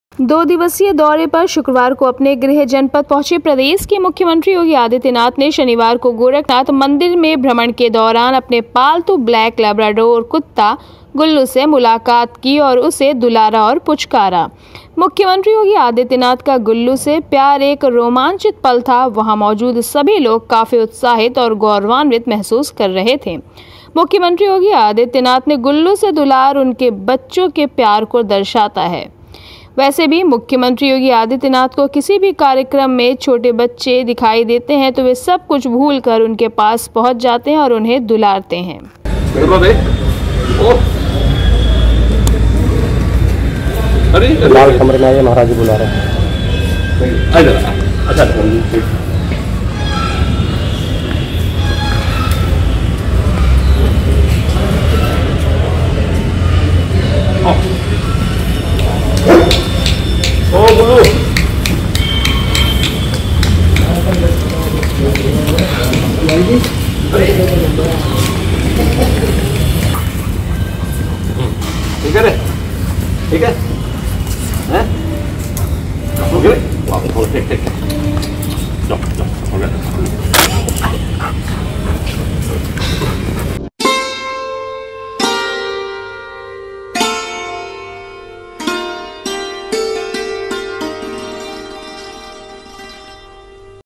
मुख्य मंत्री योगी आदित्यनाथ ने शनिवार की सुबह गोरखनाथ मन्दिर में भ्रमण के दौरान अपने पालतू ब्लैक
मुख्य मंत्री योगी आदित्यनाथ ने शनिवार की सुबह गोरखनाथ मन्दिर में भ्रमण के दौरान अपने पालतू ब्लैक लैब्राडोर कुत्ता गुल्लू को दुलारा, रोमांच से भरा रहा मुख्यमंत्री का गुल्लू से प्यार भरा पल